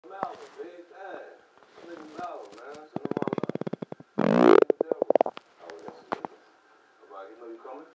1 of 3 farts from the same night...
i do not remember what i ate, but i had farts and only got 3 on video and this is #1 of 3...   enjoy...